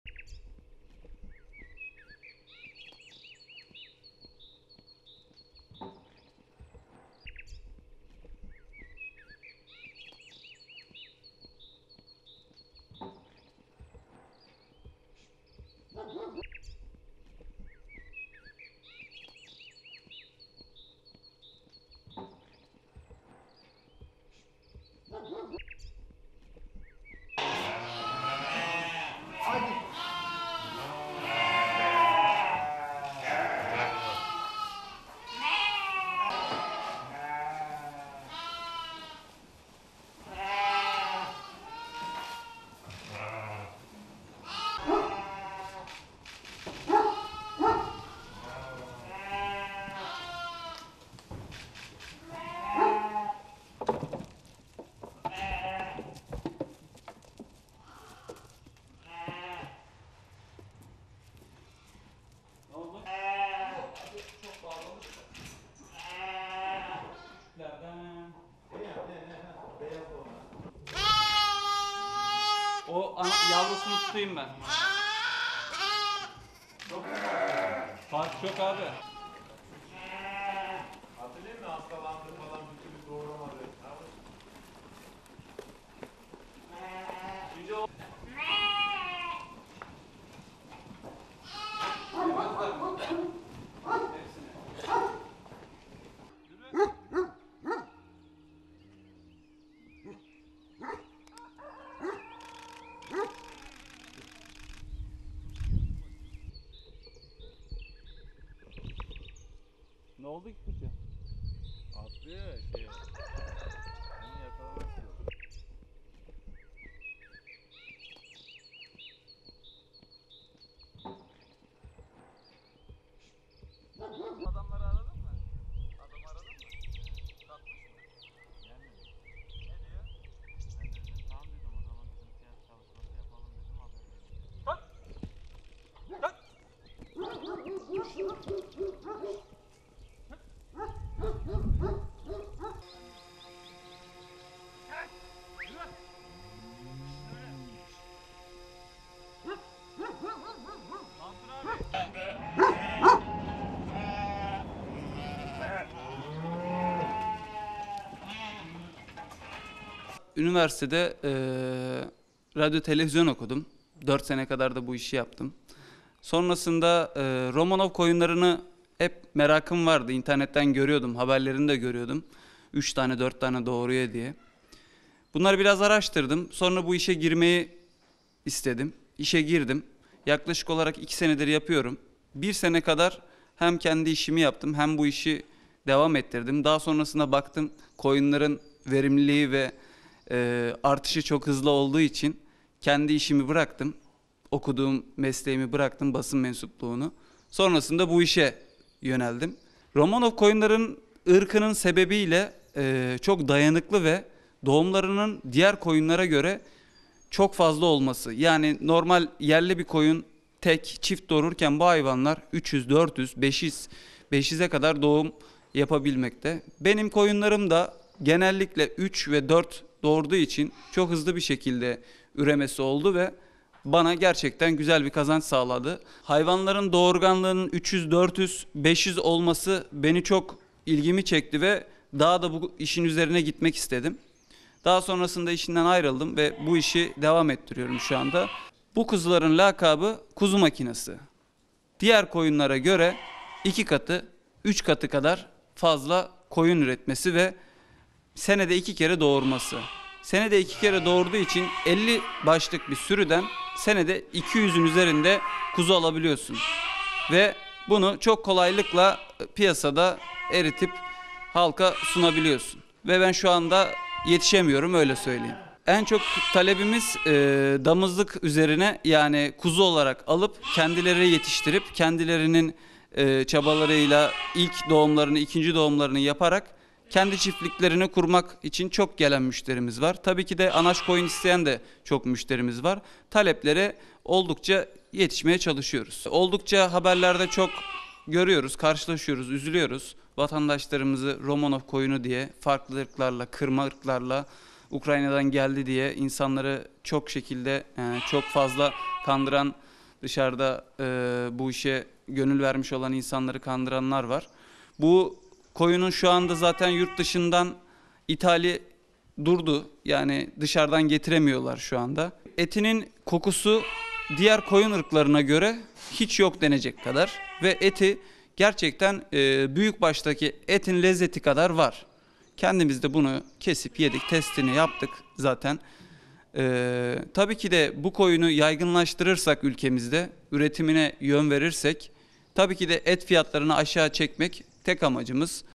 Bu koyunlar fabrika üretimi gibi doğum yapıyor
Bursa’da kendi işini yapabilmek için basın sektöründeki mesleğinden ayrılan Aydın Garip (27), beşiz doğum yapabilen ‘Romanov’ cinsi koyun yetiştirmeye başladı. Koyunların Ukrayna kökenli ve 'kuzu makinesi' olarak bilindiğini belirten Garip, "Herkes Romanov cinsi koyun beslemeye yöneldi. Taleplere yetişemiyorum" dedi.

Eskişehir Anadolu Üniversitesi Radyo Televizyon bölümünden 2015 yılında mezun olan Aydın Garip, bir süre basın sektöründe çalıştı. Garip, çiftlik kurmaya karar verdi. Çiftlikte besleyeceği hayvan için araştırma yapan Garip, yılda iki kez doğum yapan ve her doğumda üçüz, dördüz, beşiz yavrular veren ‘Romanov’ koyunlarını yetiştirmeye karar verdi. Kestel'in Gölbaşı Mahallesi'nde bulunan 10 dönümlük çiftliğine geçen yıl aldığı 4 Romanov koyunla bu işe başlayan Garip, çoklu doğumlarla kısa sürede 70 koyuna kadar ulaştı. Koyunların yetişkinleri ortalama 2 bin liradan alıcı buluyor.

‘KUZU MAKİNESİ’

Romanov koyunlarını fiziksel şartlara dayanıklı ve doğurganlıklarının diğer koyunlara göre daha fazla olması sebebiyle tercih ettiğini belirten Aydın Garip, “Yerli koyunlar tek, çift doğum yaparken bu hayvanlar 5 taneye kadar yavru verebilmekte. Benim koyunlarım da genellikle 3-4 arasında yavru verdikleri için hızlı bir şekilde üreme oldu ve güzel bir kazanç sağladı. Hayvanların doğurganlığının üçüz, dördüz, beşiz olması daha çok ilgimi çekti ve bu işin üzerine gitmek istedim. Daha sonrasında işimden ayrıldım ve bu işi devam ettiriyorum. Bu koyunların lakabı kuzu makinesi. Nedeni diğer koyunlara göre 2 katı, 3 katı kadar yavru vermesi ve senede 2 kez doğum yapması. Senede 2 kez doğurduğu için 50 başlık bir sürüden yılda 200'ün üzerinde kuzu alabiliyorsunuz. Bunu kolay bir şekilde piyasada eritip halka sunabiliyorsunuz” dedi.

‘TALEPLERE YETİŞEMİYORUM’

İnsanların artık Romanov cinsi koyun beslemeye yöneldiğini belirten Garip, “En çok talep damızlık üzerine kuzu olarak alıp, kendileri yetiştirip, kendi çabalarıyla ilk ve ikinci doğumlarını yaptırarak kendi çiftliklerini kurmak için çok gelen müşterimiz var. Anaç koyun isteyen müşterimiz de var. Şu anda taleplere yetişemiyorum” dedi.

‘ETİ KOKUSUZ VE LEZZETLİ’

Romanov cinsi koyunun etini, diğer koyunların etiyle kıyaslayan Garip, “Etinin kokusu diğer koyun ırklarına göre yok denecek kadar az. Lezzeti ise büyükbaş hayvan etinin lezzeti kadar var. Kendimiz de yedik, testini yaptık. Bu koyunu ülkemizde yaygınlaştırıp üretimine yön verirsek et fiyatlarını aşağı çekebiliriz” dedi.

‘ROMANOV CİNSİ DİYEREK BAŞKA CİNS SATIYORLAR’

Romanov koyunlarına fazla talep olması sebebiyle bazı insanların dolandırıcılığa başvurduğunu belirten Garip, “Haberlerde çok sık karşılaşıyoruz ve üzülüyoruz. Romanov cinsi diyerek bu işe gönül veren insanları kandıranlar var. Bu koyunların yurt dışından gelişi durduruldu. İthal edilmesi hiçbir şekilde söz konusu değil” dedi.